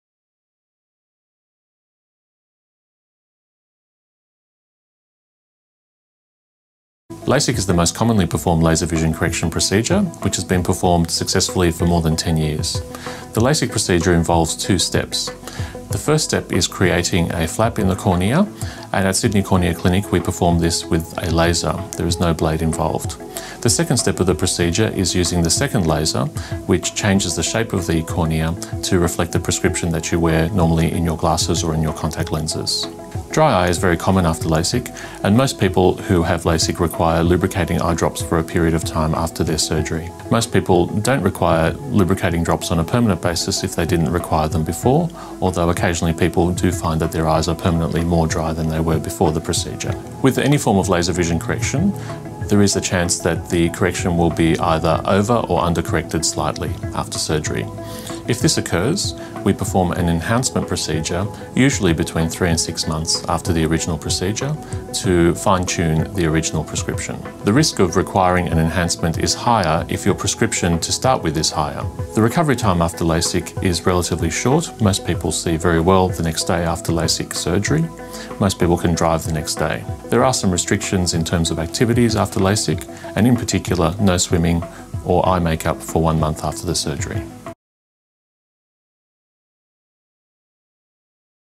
LASIK Laser Eye Surgery - Envision Eye Centre Sydney
Principle Surgeon Dr John Males explains LASIK Laser eye Surgery. He outlines who is suitable, what is involved in the procedure and the likely recovery time.